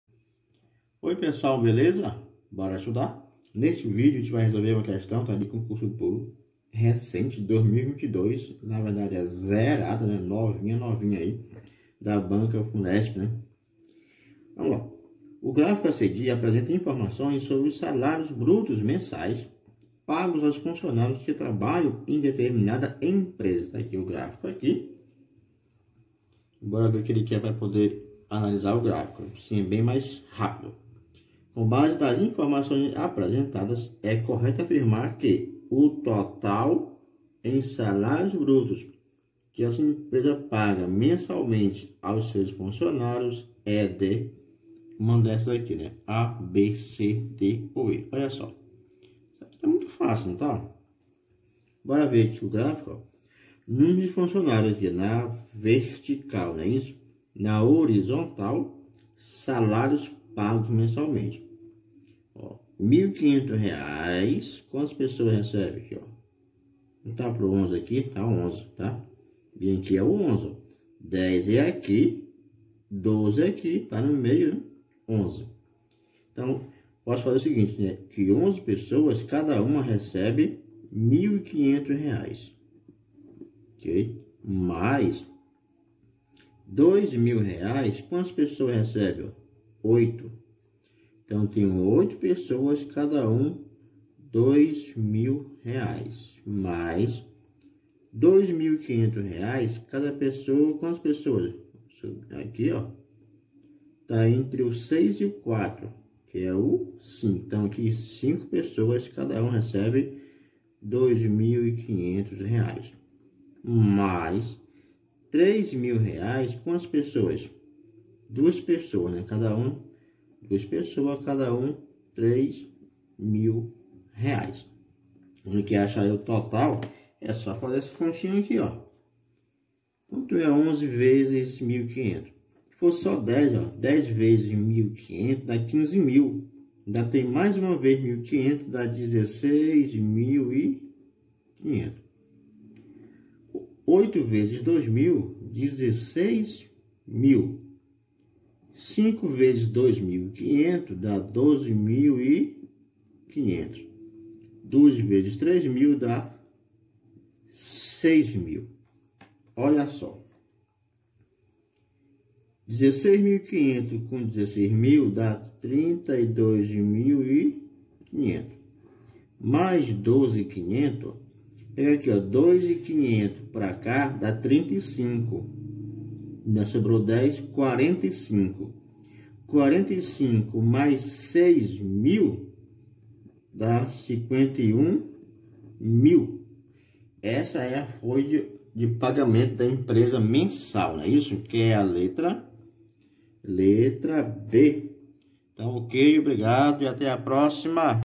Gráficos e Multiplicação e Adição em Concursos Públicos
Aula de Matemática Básica sobre leitura de Gráficos em Concursos Públicos.
Aprenda Matemática de maneira fácil e rápido com @profpipimatematica
Matemática do Básico ao Avançado é Aqui.